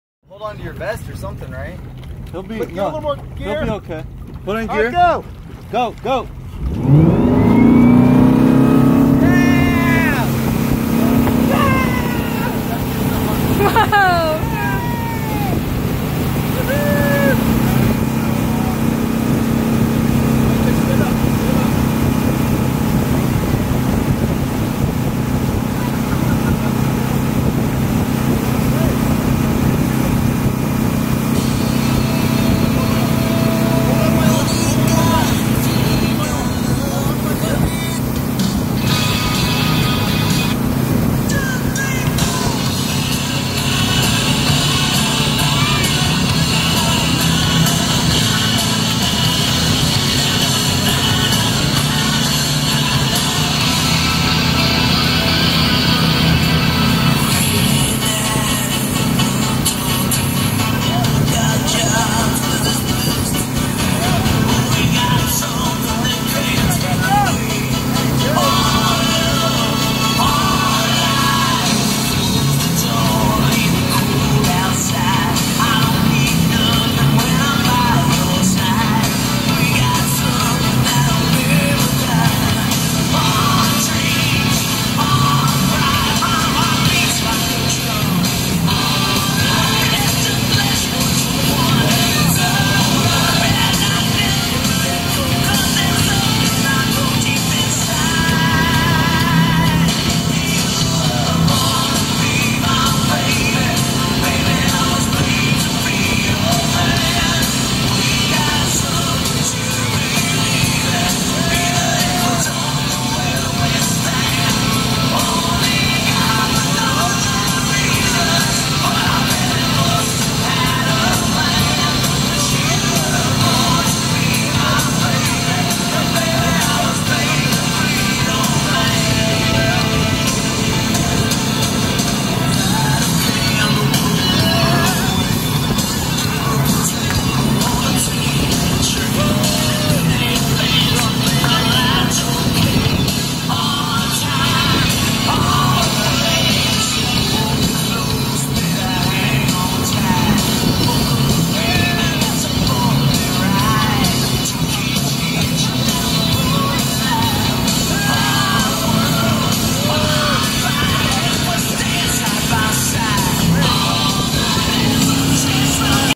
Wakesurfing with my son
Got up the first time trying it with my son